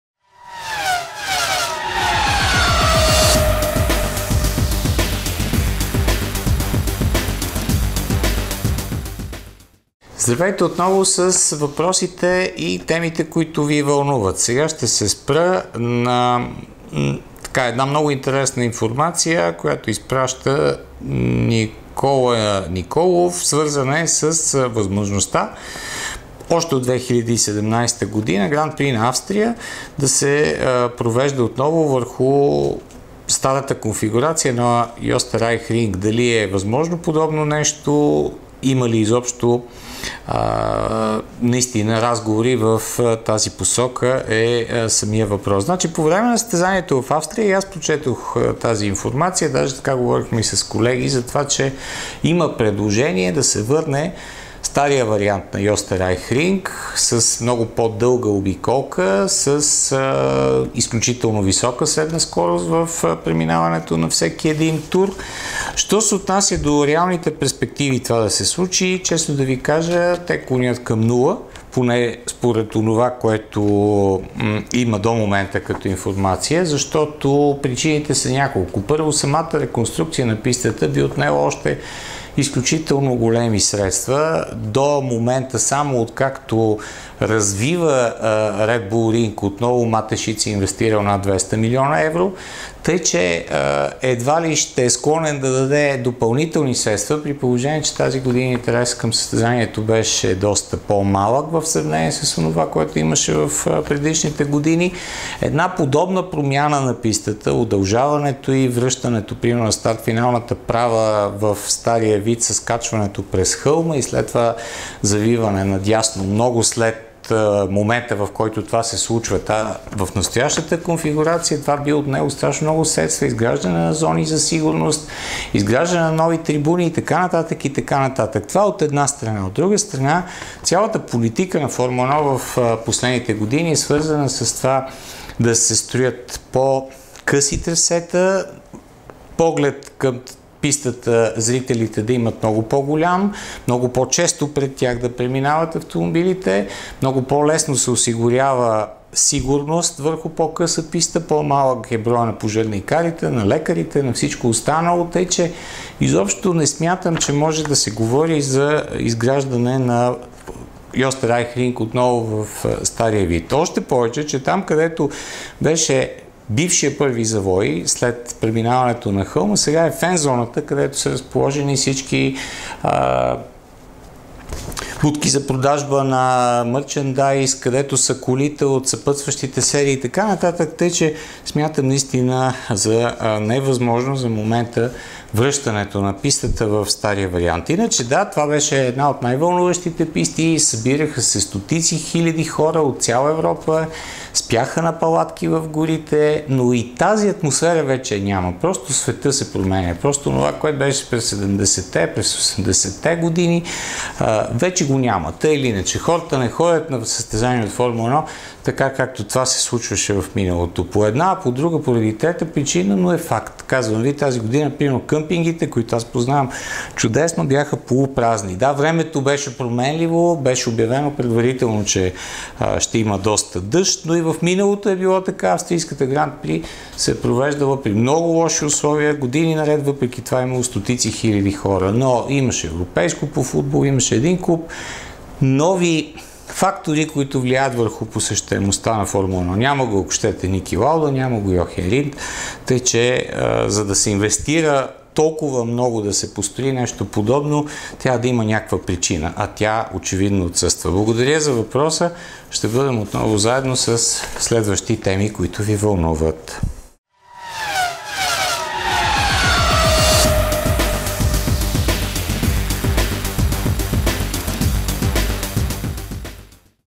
Ще се върне ли старата конфигурация на легендарната Оsterreichring?
Въпрос на Никола Николов:

Във последно време във чуждоезичните сайтове свързани със моторни спортове и Формула 1 се шуми, относно плановете на Матешиц за евентуална реконструкция на старата Оsterreichring и евентуалното провеждане на ГП на Австрия през 2017 на въпросното трасе. Моля ако имате повече информация относно това дали ще се ползва напълно оригиналната конфигурация на трасето без шикана на първи завой, както и други подробности да ги споделите със всички нас феновете на този спорт. Благодаря ви предварително за отделеното време и внимание.